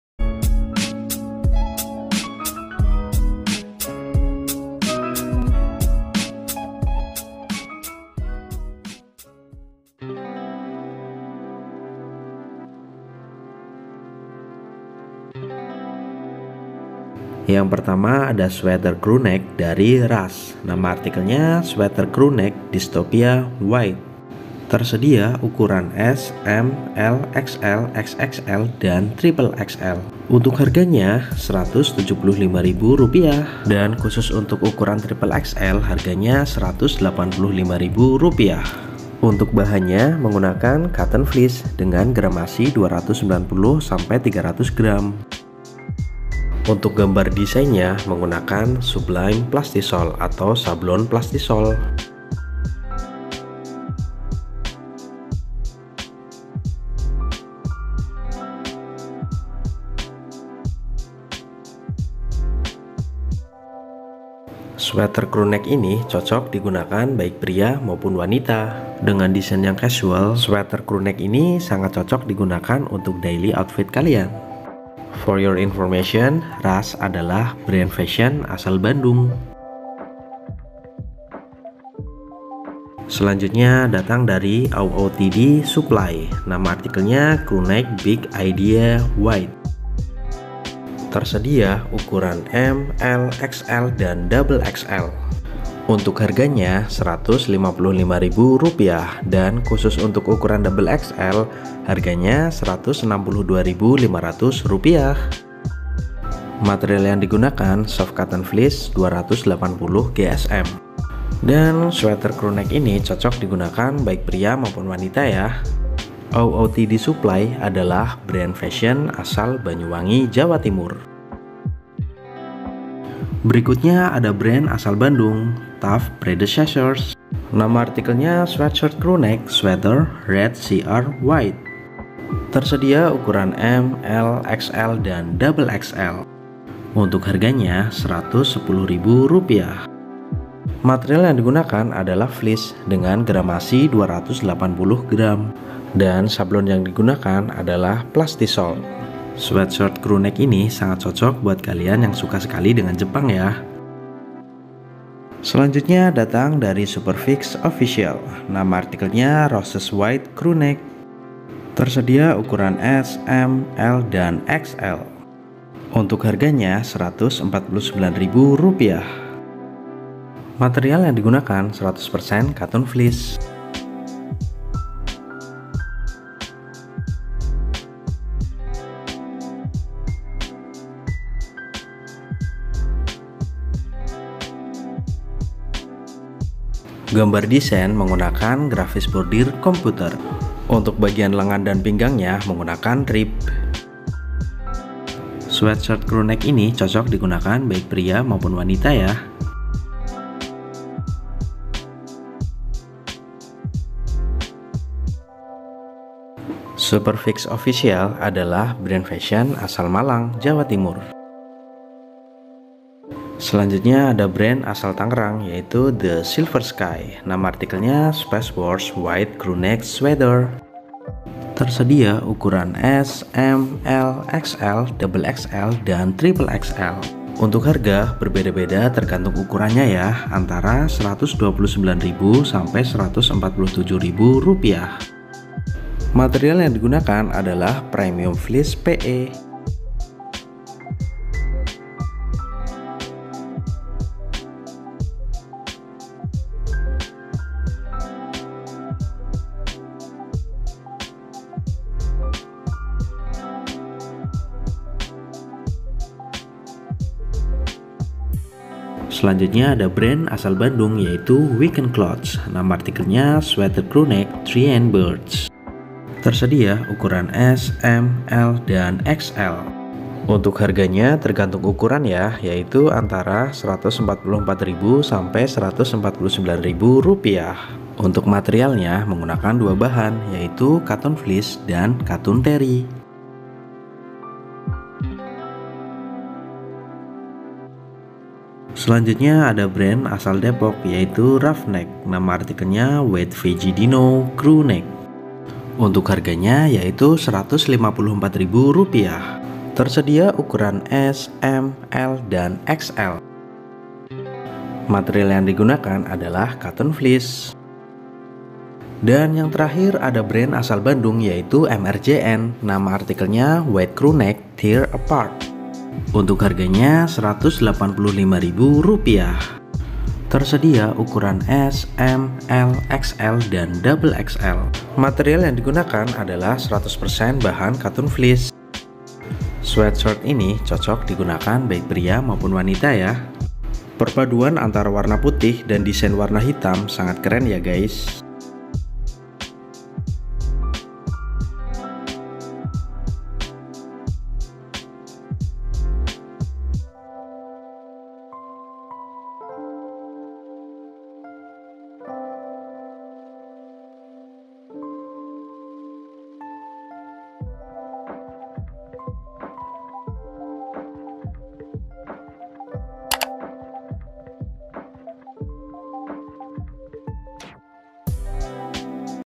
REKOMENDASI 8 SWEATSHIRT / SWEATER CREWNECK WARNA PUTIH BRAND LOKAL HARGA DI BAWAH 200 RIBU
HARGA SEWAKTU-WAKTU DAPAT BERUBAH.

LINK TOKO SHOPEE :

0:18

1:22

2:03

2:35

3:33

4:18

4:48

5:08

(Jika link tidak tersedia, kemungkinan produk sudah habis)